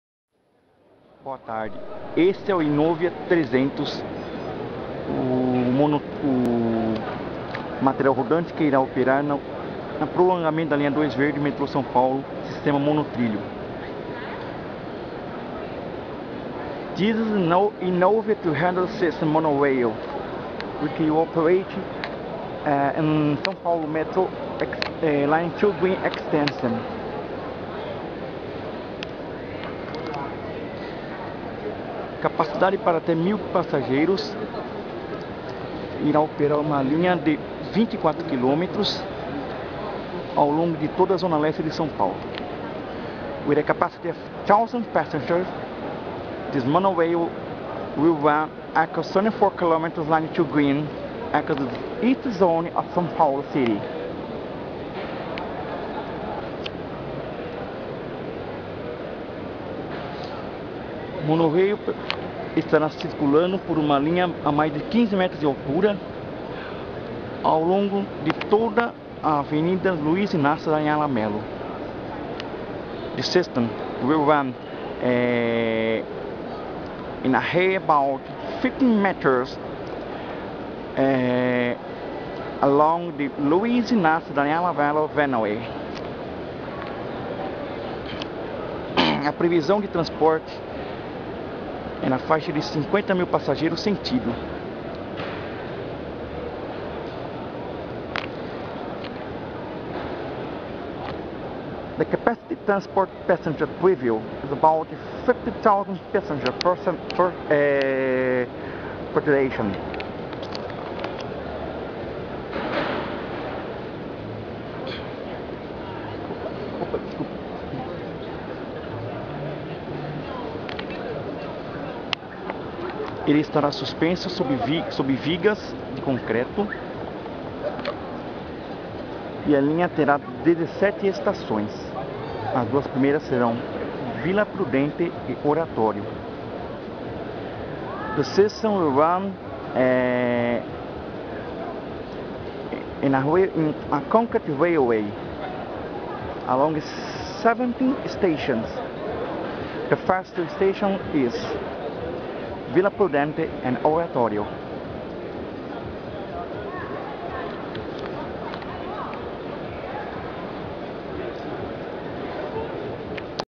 Innovia 300 Monorail - NT 2011 08/11/2011
Explicação sobre o modelo de monotrilho que a Bombardier usará na expansão da linha 2 verde do Metrô
Explanation about the monorail system that Bombardier will use in the Line 2 grenn from São Paulo Metrô
Explicación acerca del modelo de monotrillo que la Bombardier utilizará en el Metrô de São Paulo en la línea 2 verde